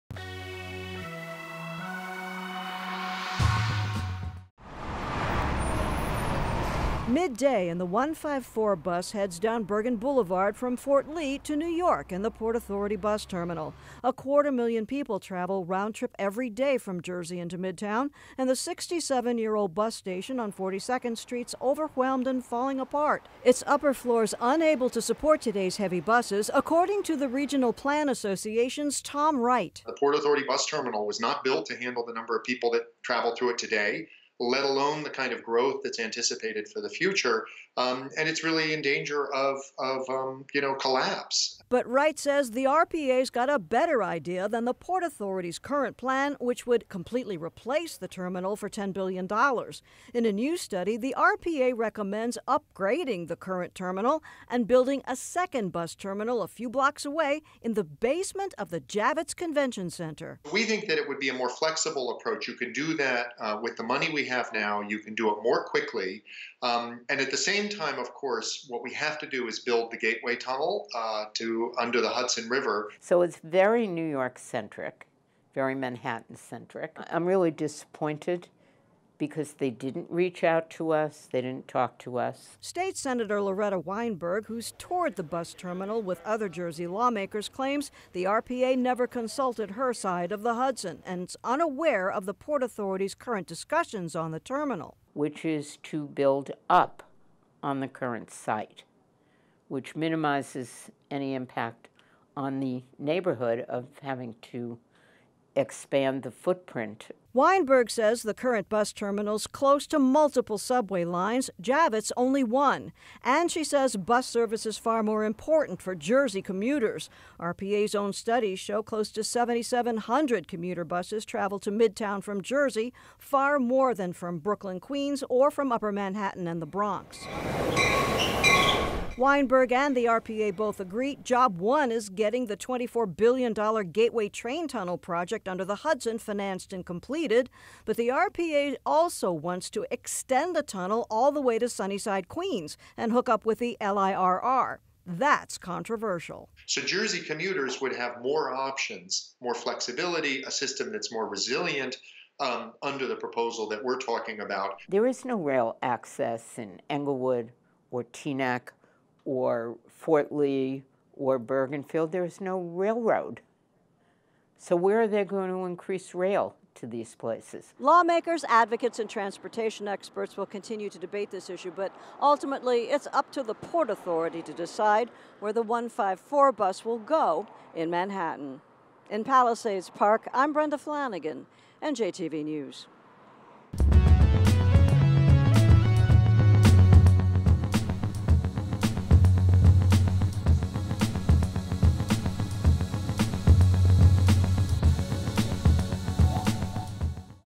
Regional Plan Association proposes revamp to transit system across the Hudson
Senators critical of proposal to build bus terminal at Javits Center instead of revamping current Port Authority Bus Terminal.